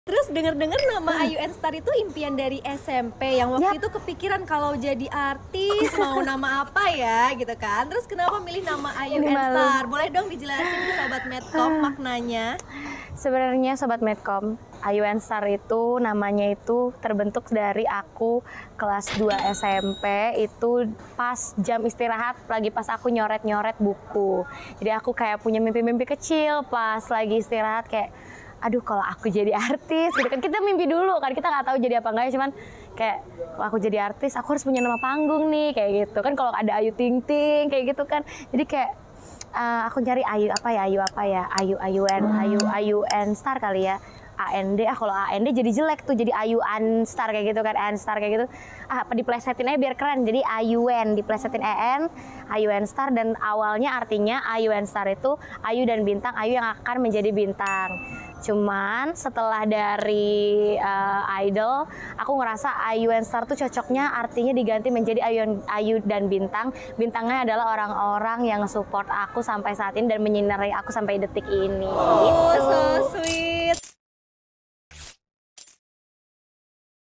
NAMA AYUENSTAR TERNYATA IMPIAN DARI SMP
Penyanyi jebolan Indonesian Idol 2018, Ayuenstar yang akrab disapa Ayu ini ternyata memiliki impian jadi artis sejak SMP. Bahkan nama panggung Ayuenstar sendiri juga sudah ia persiapkan.